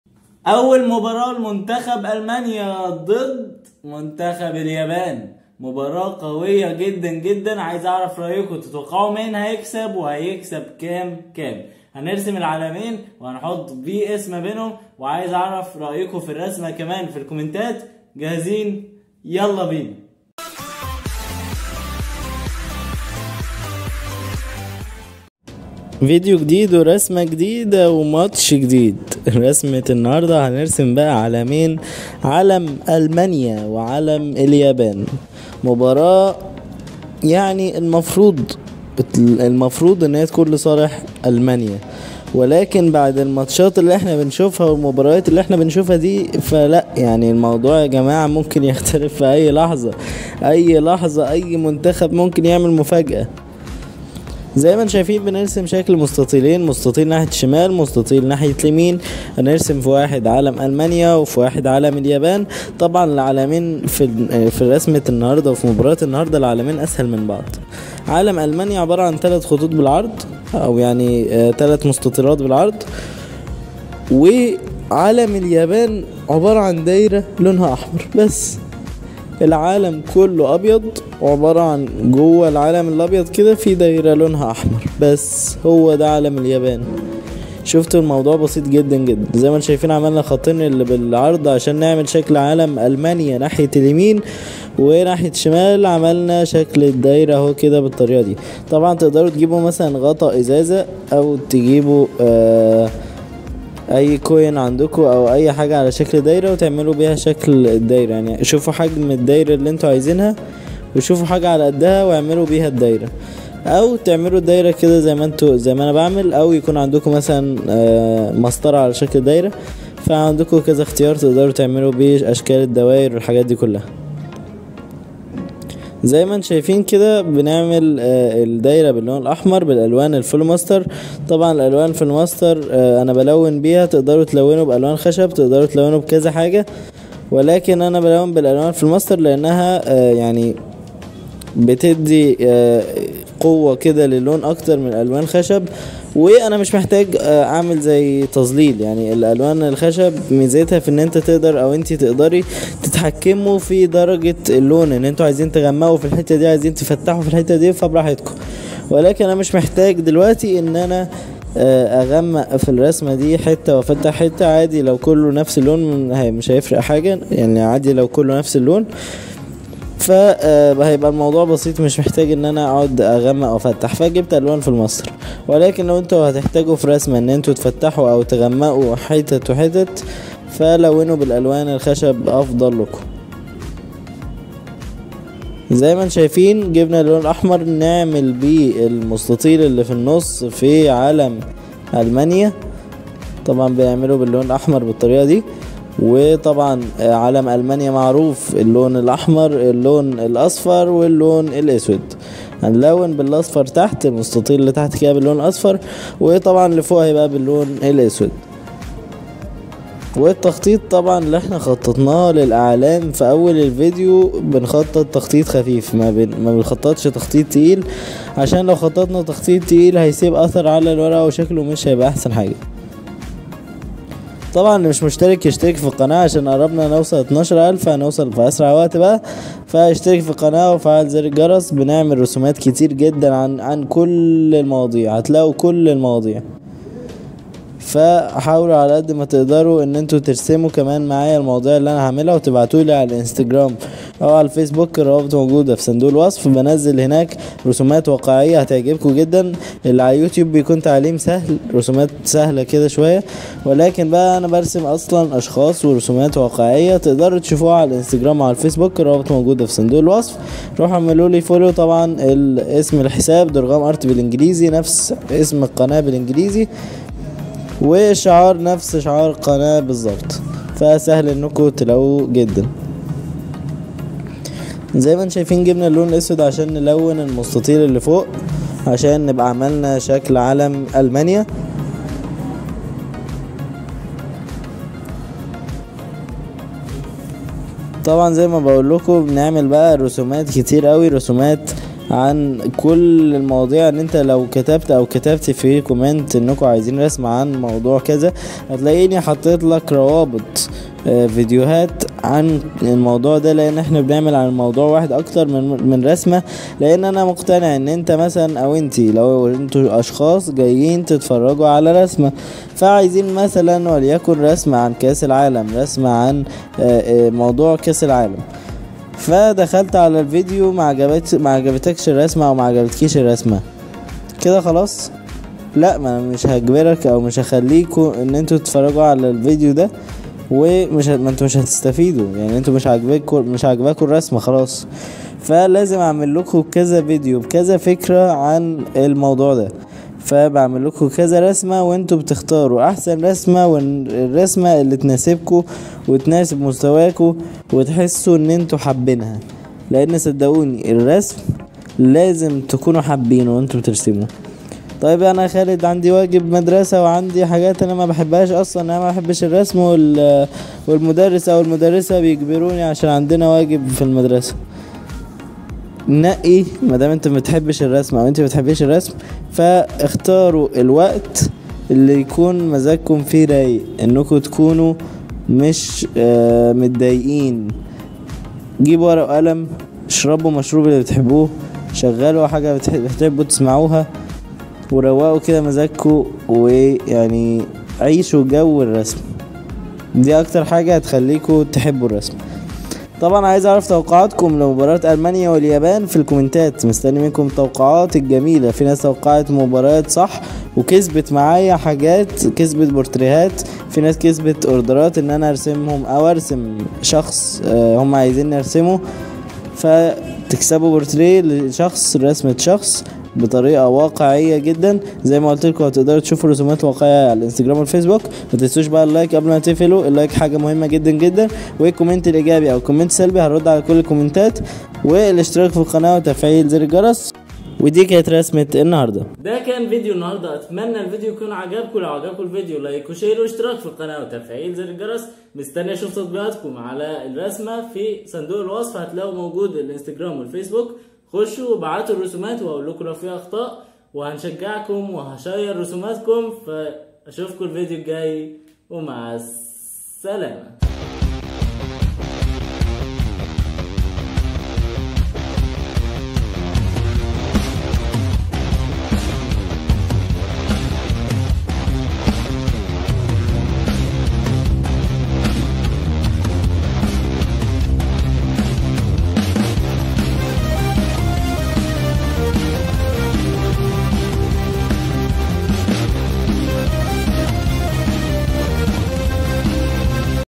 ألمانيا ضد اليابان | رسم المانيا ضد اليابان في كأس العالم 2022
ألمانيا ضد اليابان
تعلم رسم عن ألمانيا ضد اليابان. في هذه الرسمة سنتعلم كيف ارسم المانيا واليابان وكيف ارسم عن ألمانيا ضد اليابان للمبتدئين. رسم المانيا ضد اليابان للمبتدئين والأطفال وبطريقة سهلة.

● برنامج ( Adobe Premiere 2021 )
ــــــــــــــــــــــــــــــــــــــــــــــــــــــــــــــ
ــــــــــــــــــــــــــــــــــــــــــــــــــــــــــــــ
ــــــــــــــــــــــــــــــــــــــــــــــــــــــــــــــ
ــــــــــــــــــــــــــــــــــــــــــــــــــــــــــــــ
ــــــــــــــــــــــــــــــــــــــــــــــــــــــــــــــ
ــــــــــــــــــــــــــــــــــــــــــــــــــــــــــــــ
ــــــــــــــــــــــــــــــــــــــــــــــــــــــــــــــ
ــــــــــــــــــــــــــــــــــــــــــــــــــــــــــــــ
فيديوهات جميلة جدا
ــــــــــــــــــــــــــــــــــــــــــــــــــــــــــــــ
ــــــــــــــــــــــــــــــــــــــــــــــــــــــــــــــ
ــــــــــــــــــــــــــــــــــــــــــــــــــــــــــــــ
ــــــــــــــــــــــــــــــــــــــــــــــــــــــــــــــ
ــــــــــــــــــــــــــــــــــــــــــــــــــــــــــــــ
 ــــــــــــــــــــــــــــــــــــــــــــــــــــــــــــــ
ــــــــــــــــــــــــــــــــــــــــــــــــــــــــــــــ
ــــــــــــــــــــــــــــــــــــــــــــــــــــــــــــــ
ــــــــــــــــــــــــــــــــــــــــــــــــــــــــــــــ
ــــــــــــــــــــــــــــــــــــــــــــــــــــــــــــــ
هاشتاج درغام ارت